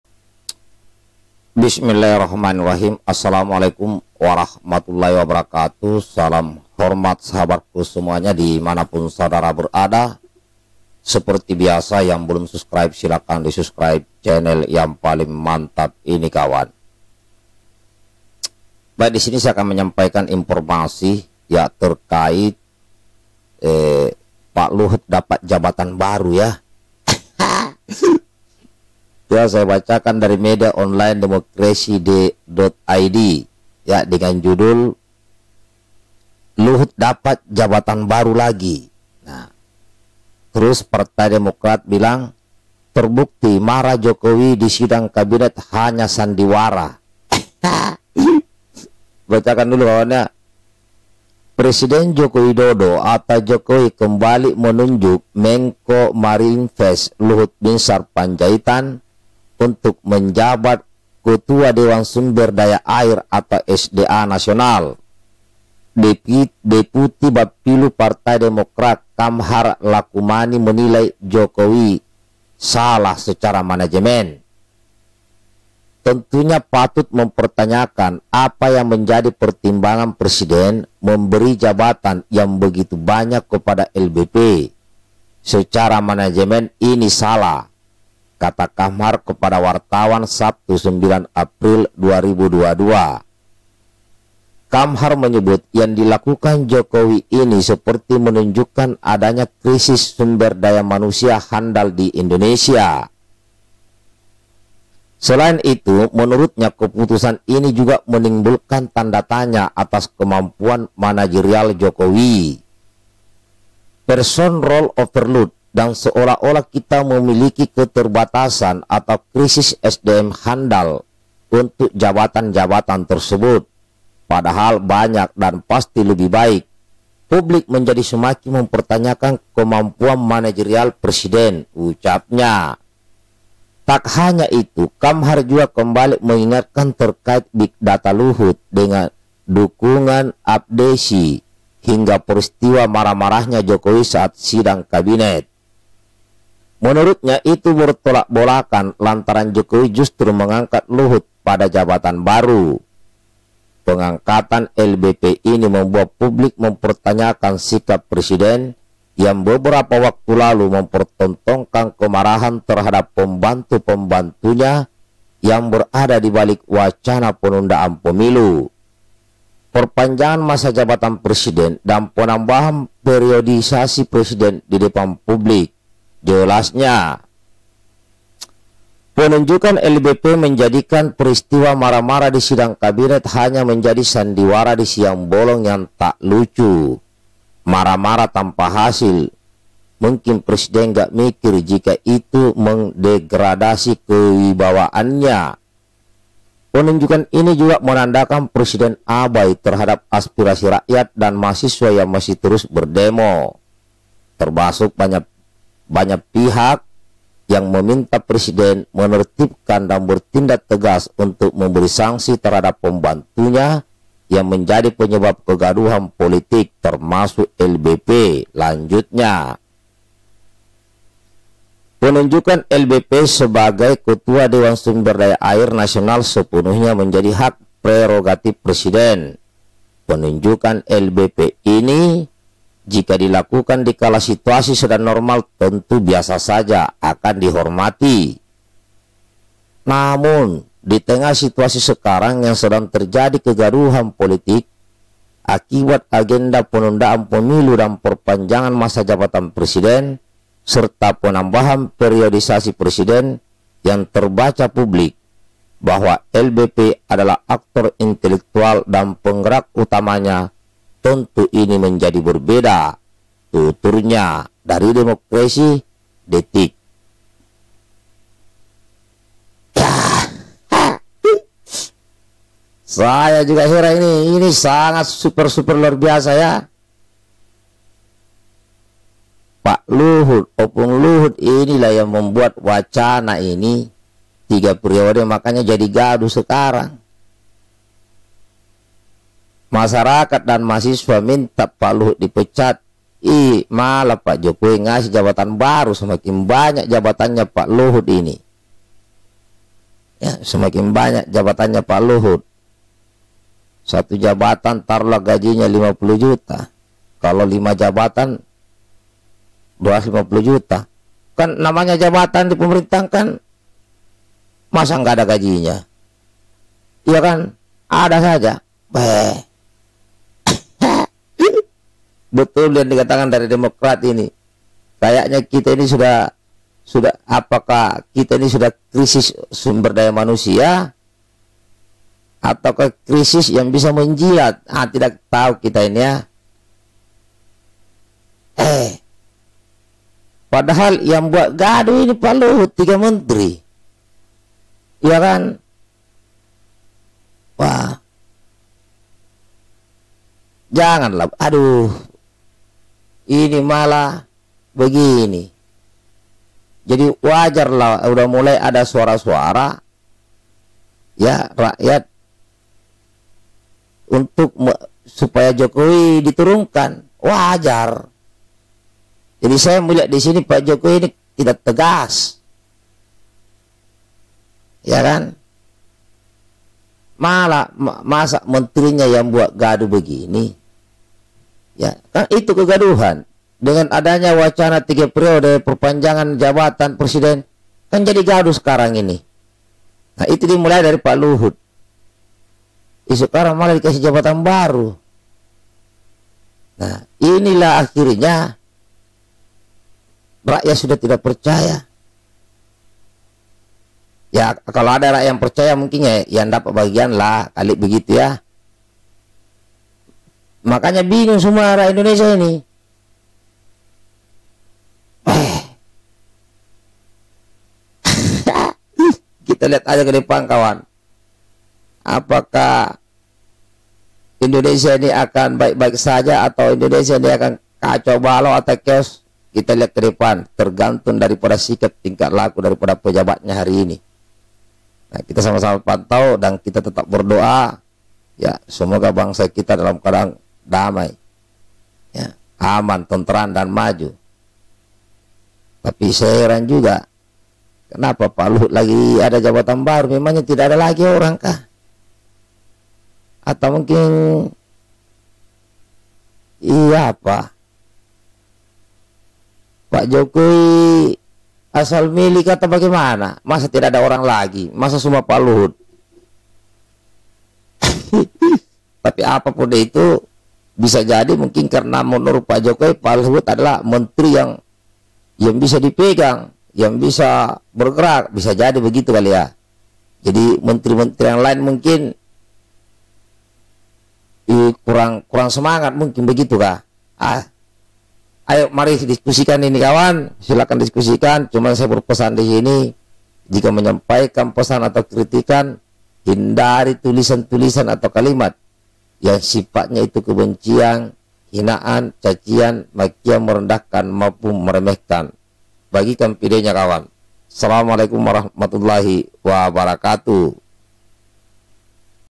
HAHAHA Pak LUHUT Bukannya DIPECAT SELAKU MENTERI Krn TELAH BUAT GADUH, MALAH DI BERI JABATAN BARU.
BUKANNYA DIPECAT SELAKU MENTERI KARENA TELAH BUAT GADUH
TAPI MALAH DI BERI JABATAN BARU OLEH PRESIDEN.
MPS: Sangat Super Luar Biasa

Aamiin.

TTD